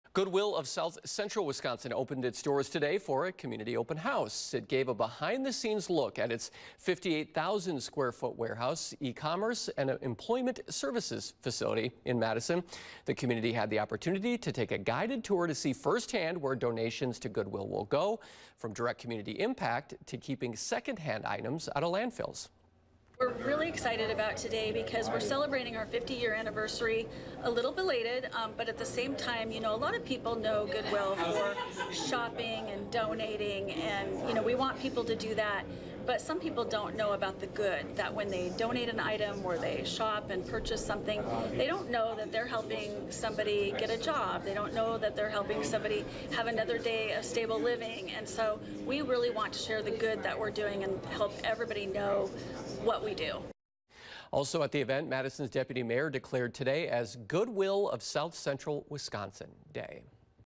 Goodwill hosts community open house for 50th anniversary
Goodwill of South Central Wisconsin opened its doors for a community open house Friday.

The organization gave a behind-the-scenes look at its 58,000-square-foot warehouse, e-commerce, and employment services facility in Madison.

The community had the opportunity to take a guided tour to see firsthand where donations to Goodwill go, from direct community impact to keeping secondhand items out of the landfill.